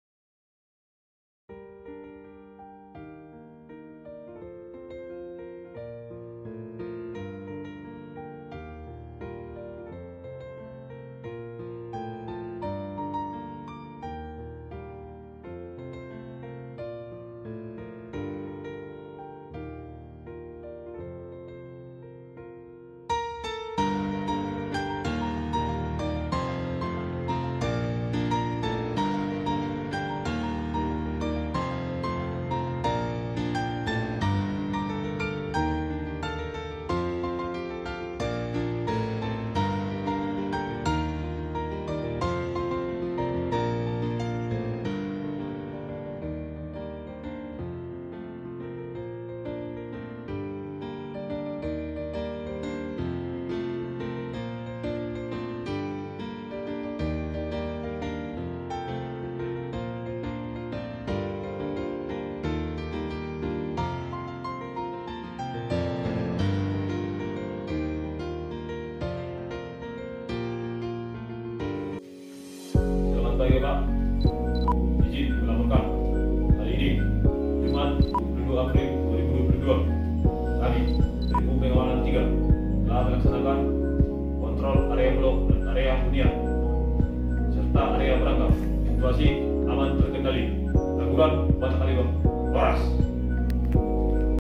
Giat Troling Rupam malam Lapas Pangururan Kanwil Kemenkum Sumut
Samosir 22 April 2022
Petugas Pengamanan Malam Lapas Kelas III Pangururuan Kanwil Kemenkumham Sumut Laksanakan Kotrol Keliling Area Brandgang dan Blok Hunian
Dalam pekerjaan tugas untuk meningkatkan keamanan dan ketertiban di malam hari, Lapas Kelas III Pangururan Kanwil Kemenkumham Sumut melaksanakan kontrol keliling (Trolling) pada sekitar area brandgang serta kawasan blok hunian WBP. Kegiatan tersebut dilaksanakan oleh Regu Pengamanan Piket Malam dengan penuh semangat dan tentunya selalu waspada terhadap sekecil apapun kemungkinan terjadinya gangguan kamtib.
Kegiatan berjalan dengan situasi aman dan terkendali.
SELURUH LAYANAN LAPAS PANGURURAN GRATISS TANPA DIPUNGUT BIAYA
LAGURAN
BATAK KALI BAH! 💪🏽